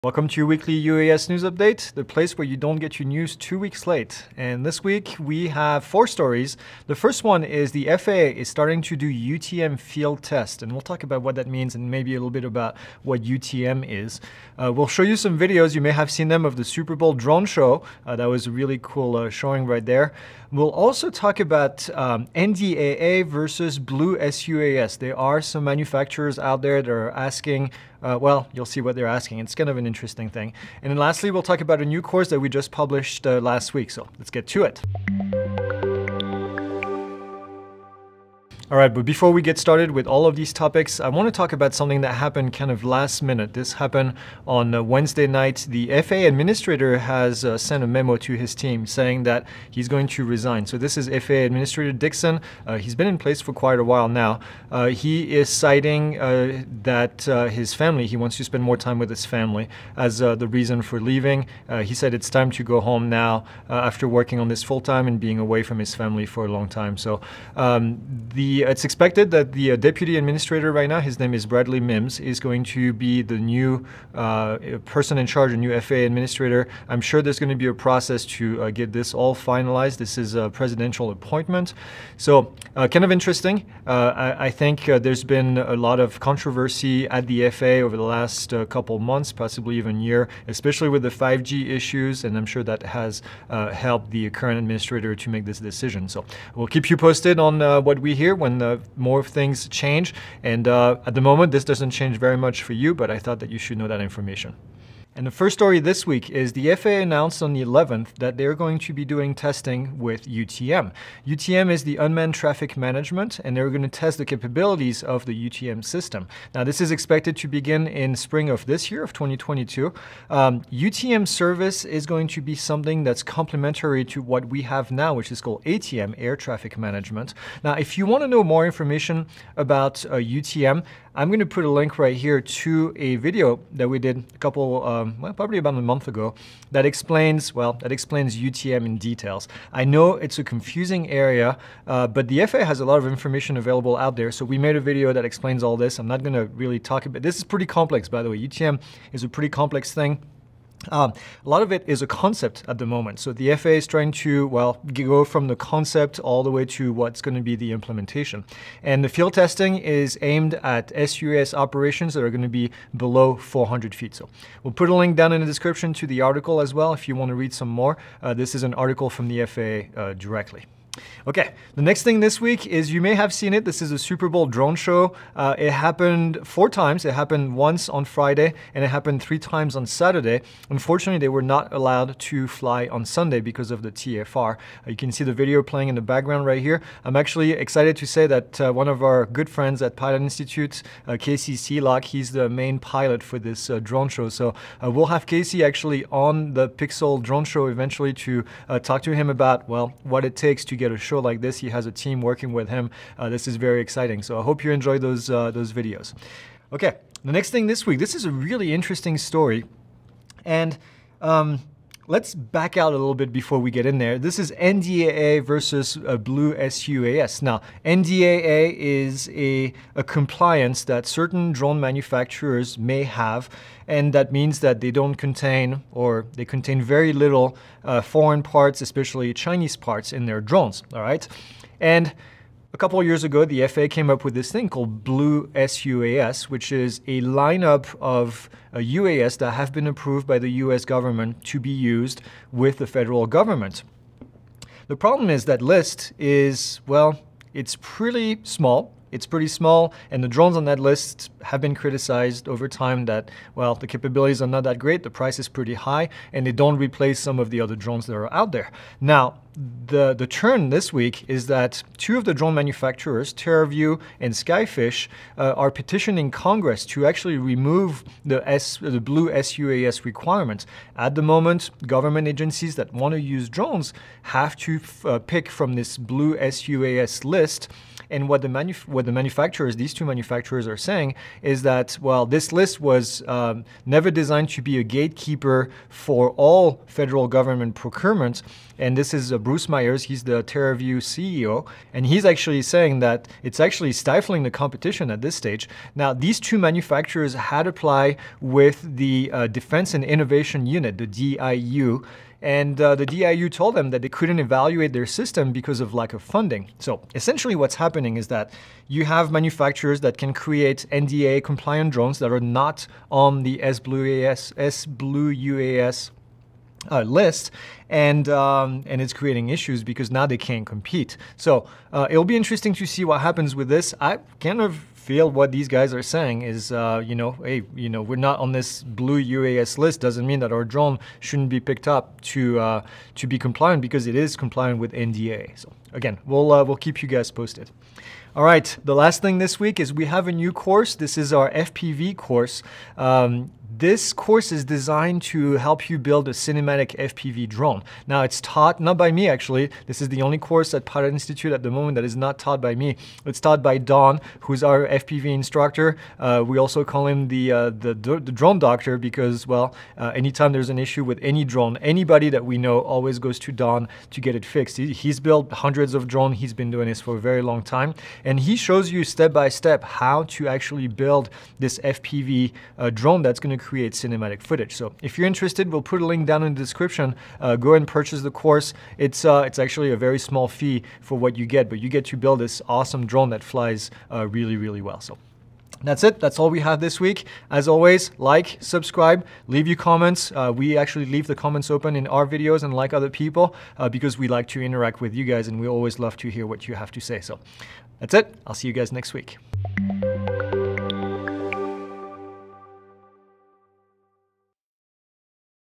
Drone News: FAA Administrator Resigns, UTM Field Test, Superbowl Drone show, NDAA vs Blue UAS
This week in drone news:  FAA Administrator has announced he will be resigning, the FAA announced this week they will begin Unmanned Traffic Management field testing, the Superbowl drone show, more concerns over blue UAS in comparison to the NDAA, and a new FPV course.

00:00 Introduction
00:38 FAA Administrator Resigns
01:44 FAA Starts UTM Field Test
02:50 Superbowl Drone Show
03:27 NDAA v. Blue UAS
05:37 New Course!

Friday the 11th, the FAA announced the plan to begin testing unmanned traffic management capabilities.
Testing will start in the spring of 2022.
UTM service will be complementary but separate from ATM services
This field testing is aimed at sUAS operations, generally, below 400’.

The NFL put on a drone show for the Super Bowl.
This show was shown 4 times, once the Friday prior and three times on Saturday.
There were none on Sunday due to the TFR.

Two NDAA compliant drones manufacturers, Terraview and Skyfish are petitioning congress to remove the blue sUAS requirement.
These two manufactures claim the Blue UAS program is causing damage to NDAA compliant manufacturers.
Both manufacturers applied for blue UAS, but were told the Defense Innovation Unit (DIU) lacked the funding to evaluate their systems.
“The Blue sUAS list was never designed to be a gate-keeper for federal government procurement,” says Bruce Myers, Terraview CEO.  “It’s actually stifling competition… The DIU never intended the Blue sUAS list to be the be all end all. ”

Check our our new FPV course!
This is a step-by-step guide to building a cinematic FPV drone, taught by Don, our FPV instructor.
Don goes over what parts to use, how to assemble, and configure the drone to get those awesome dynamic FPV shots.

The FAA administration released a memo stating:
“Over the past several years, my family has been a source of tremendous encouragement, strength and support. Nevertheless, after sometimes long and unavoidable periods of separation from my loved ones during the pandemic, it is time to devote my full time and attention to them. As I wrote in my letter to President Biden, it is time to go home.”
Administrator Dickson said he made the decision in light of growing extended family and time away from his family.
It’s expected Deputy Administrator Bradley Mims will take over Dickson’s duties.